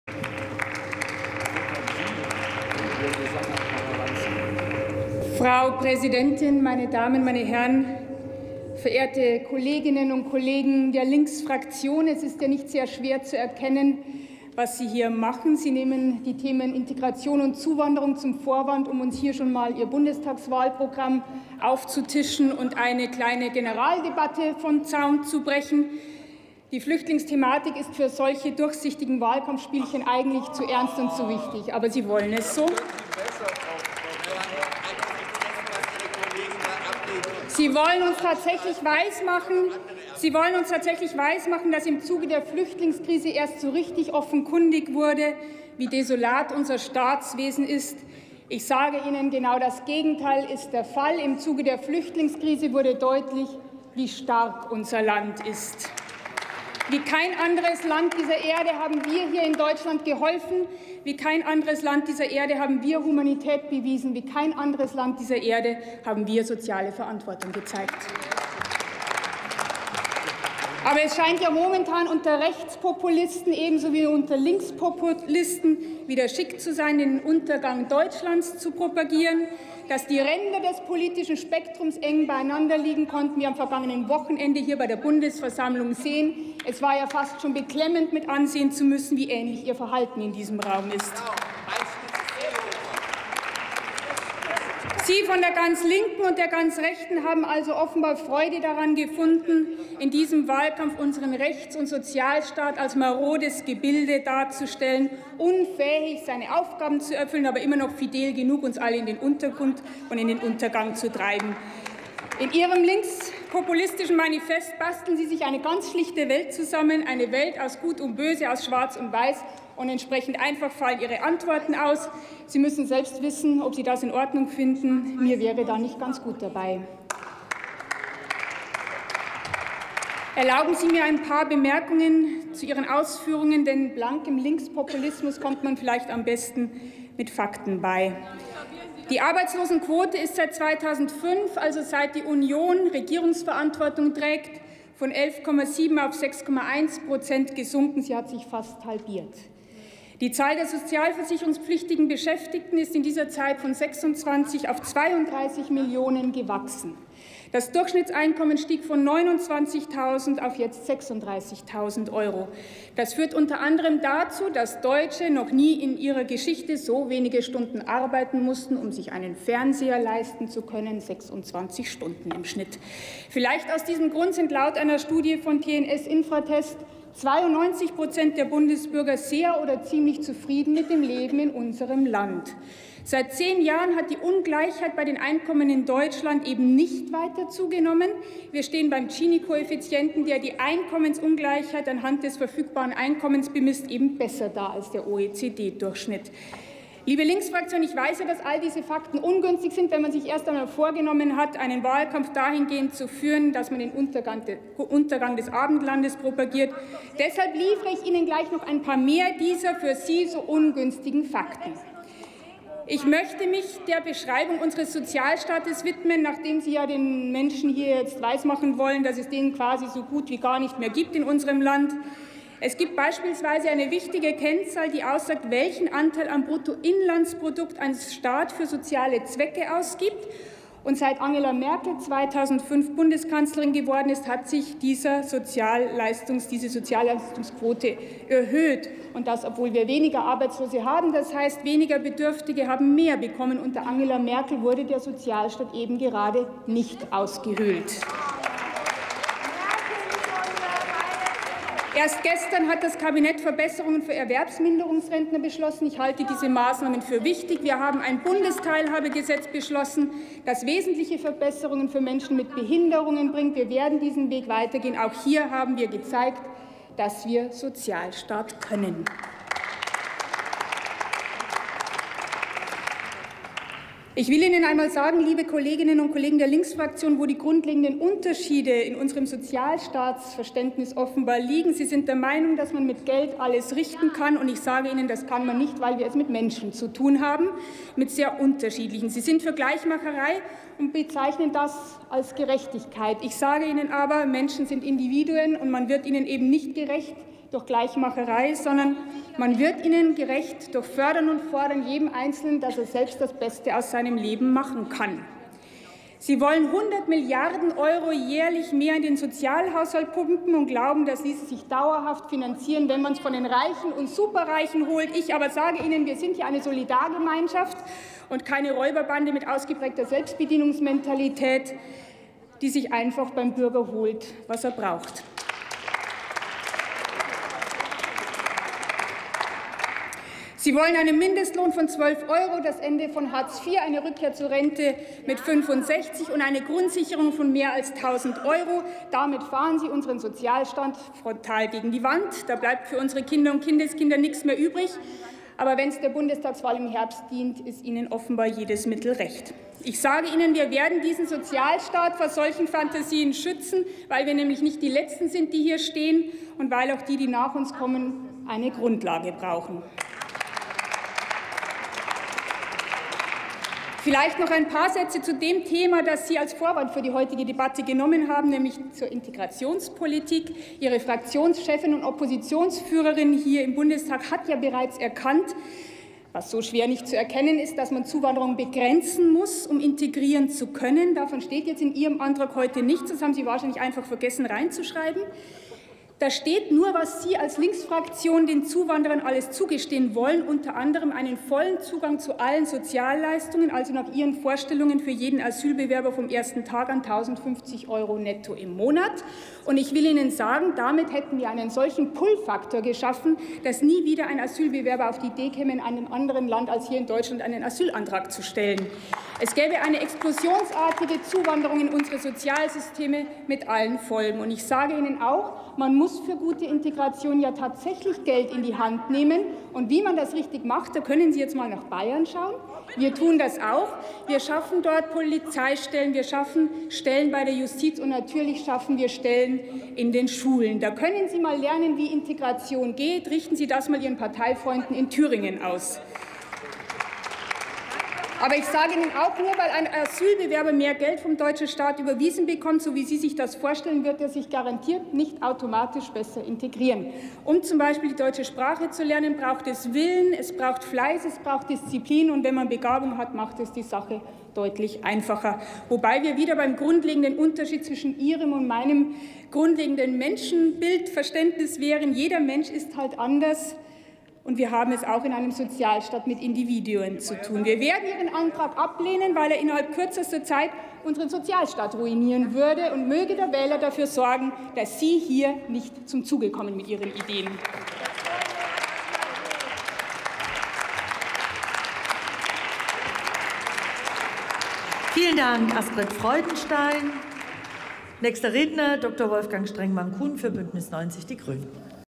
Astrid Freudenstein: Integrationspolitik [Bundestag 16.02.2017]
Eine erfolgreiche Integrationspolitik erfordert eine soziale Offensive für alle
Drucksache 18/9190

18. Deutscher Bundestag
218. Plenarsitzung
Tagesordnungspunkt: Integrationspolitik
Redner: Freudenstein, Dr. Astrid, CDU/CSU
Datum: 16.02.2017
Plenarprotokoll:
UVID:7073491